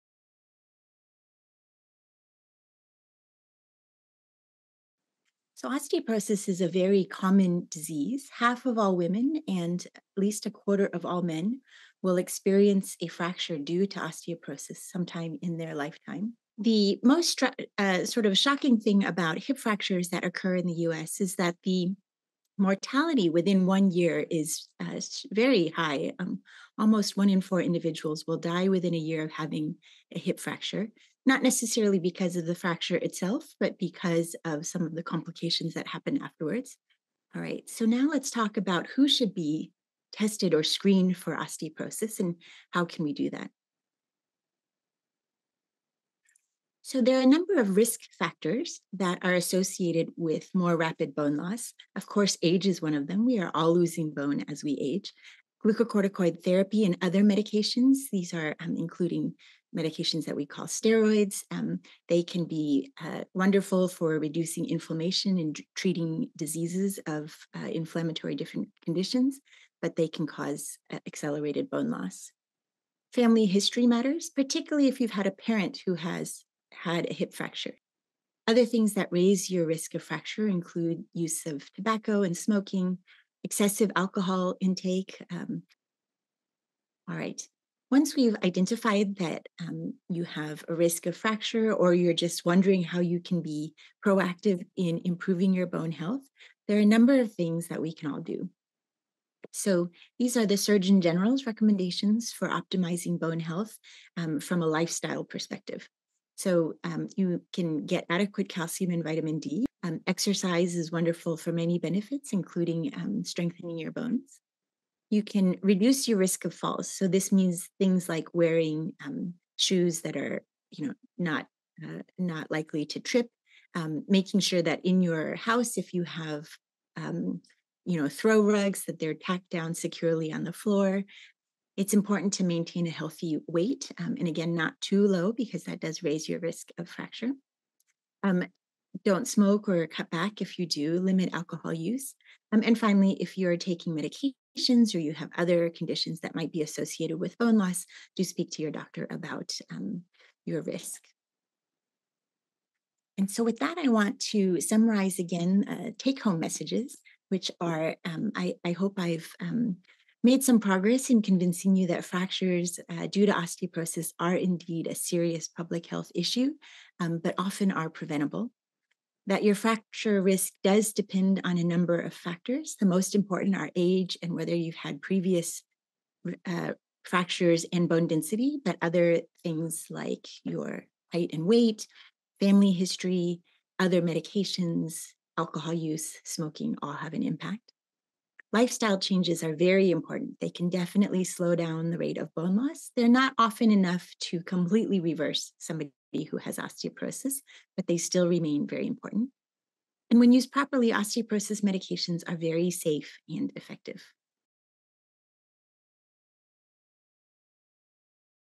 Prevention and Treatment of Osteoporosis in Asian Americans (1-minute version)
Over 50% of women and 25% of men over the age of 50 will sustain a fragility fracture due to osteoporosis. Joy Wu, MD, PhD, will talk about risk factors, prevention and treatment options, as well as common questions about lifestyle, dietary supplements, and medications.

Joy Wu, MD, PhD, is a board-certified endocrinologist who specializes in treating osteoporosis and other bone and mineral diseases, including primary hyperparathyroidism, vitamin D deficiency, Paget's disease and fibrous dysplasia. Dr. Wu is Chief of the Division of Endocrinology, Gerontology and Metabolism and Vice Chair of Basic Science in the Department of Medicine at Stanford